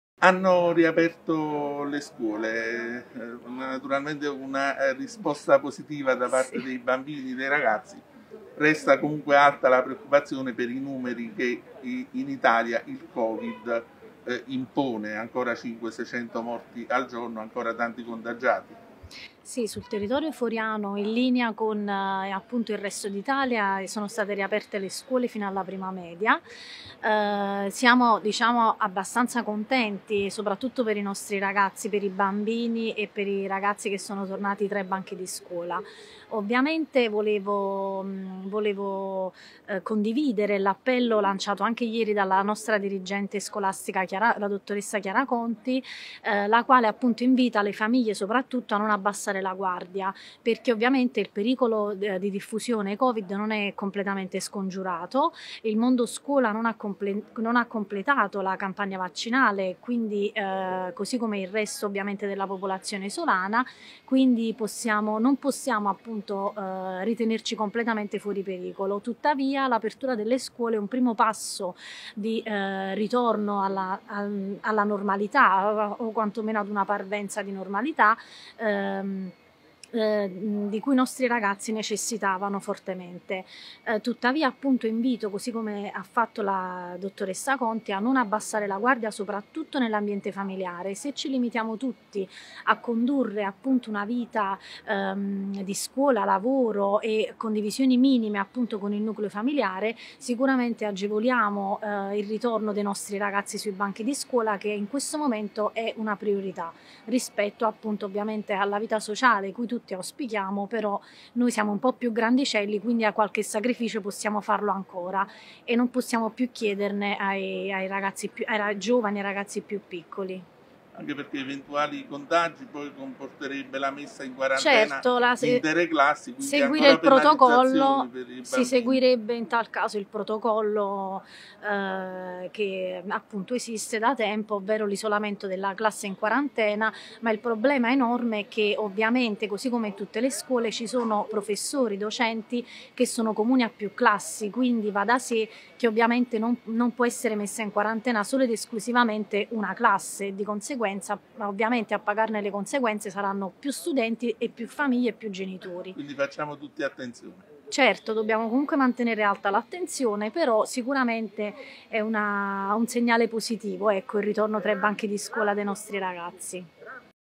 Forio (NA) - BENE LA RIAPERTURA DELLE SCUOLE (09.04.21)
- Forio (NA) - “BENE LA RIAPERTURA DELLE SCUOLE MA RESTI ALTA LA GUARDIA PER EVITARE L’ULTERIORE DIFFONDERSI DEL CONTAGIO DA COVID-19”, A NUVOLA TV L’ACCORATO APPELLO DI MANUELA ARTURO ALLA CITTADINANZA DI FORIO A CONTINUARE AD AVERE COMPORTAMENTI RESPONSABILI
Ritorniamo a parlare di riapertura delle scuole e stavolta lo facciamo con Manuela Arturo, Consigliere comunale di Forio con delega alla scuola. “Bene il ritorno dei ragazzi tra i banchi di scuola ma deve essere sempre molto alta la guardia da parte delle famiglie per evitare una nuova recrudescenza dei contagi”, ha affermato tra l’altro la Arturo. (09.04.21)